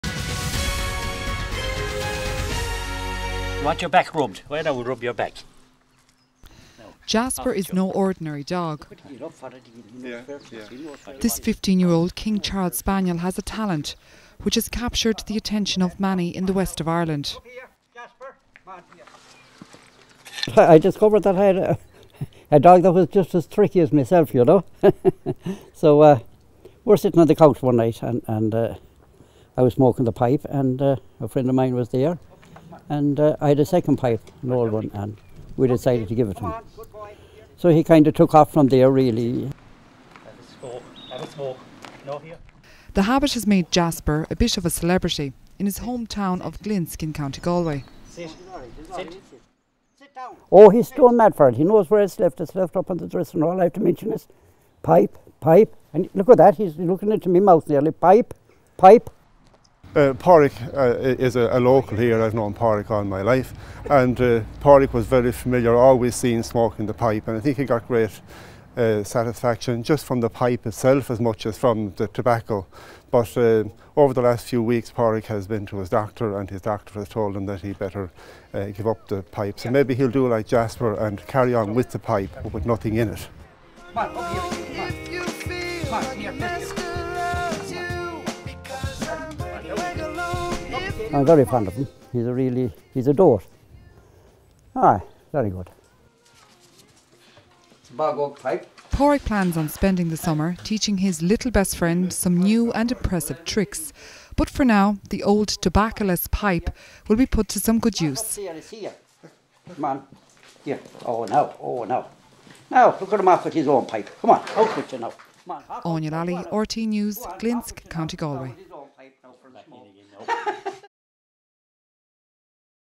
Galway Dog 'Smokes' a Pipe
This Galway dog amuses his owner and neighbours with his fondness for a pipe.